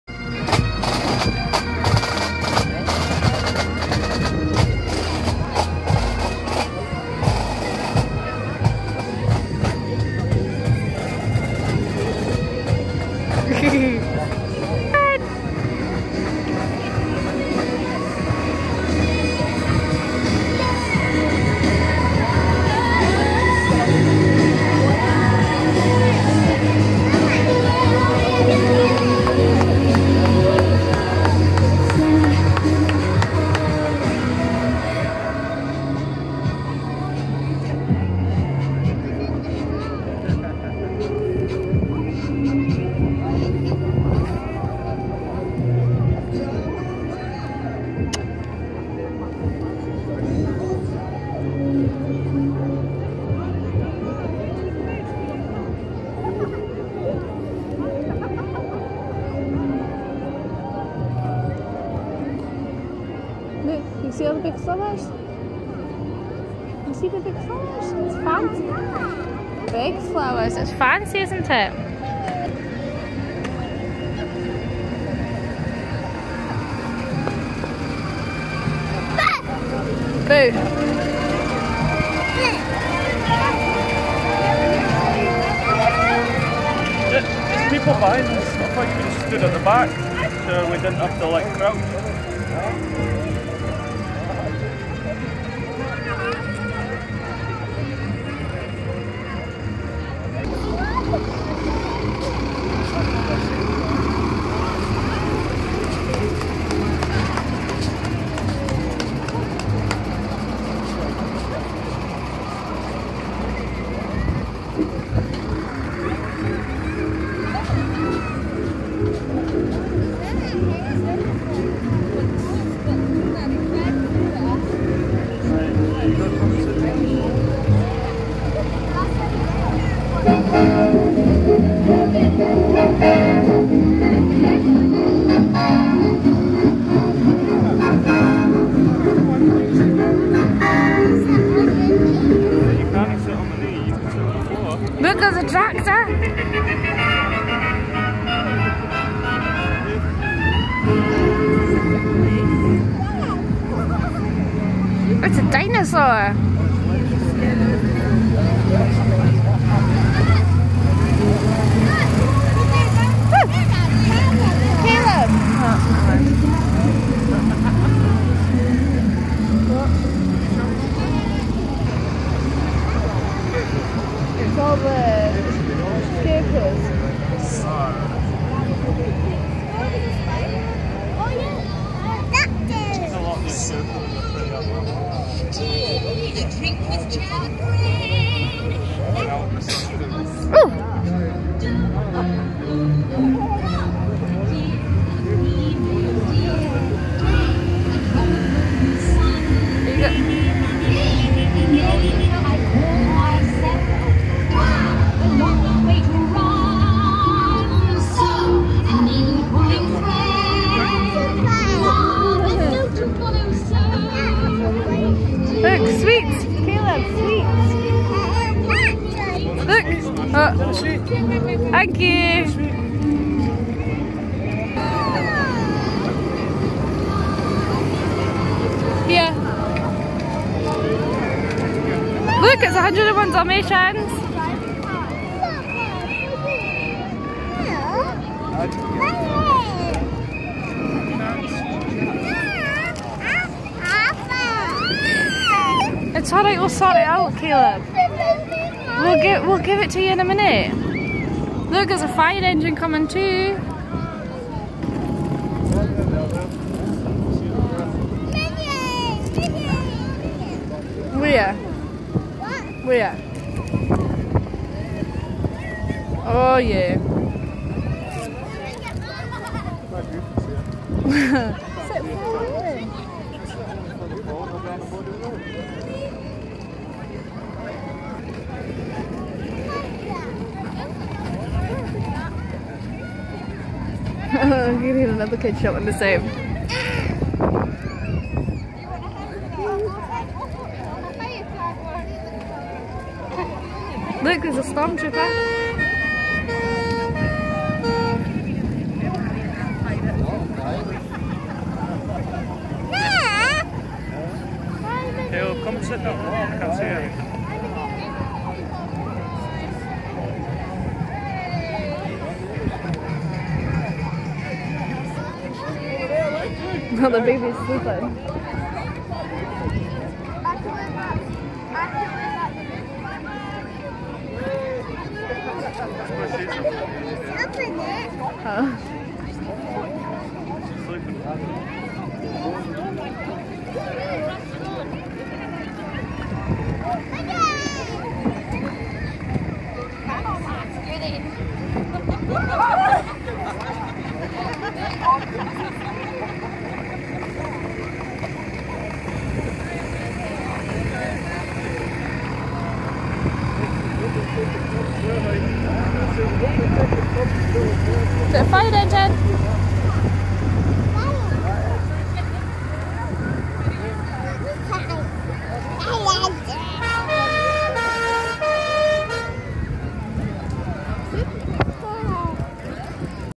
Gretna gala Parade 2016
Gretna's parade 2016.
I missed recording Just one car infront of the band with Tibby Marshal in.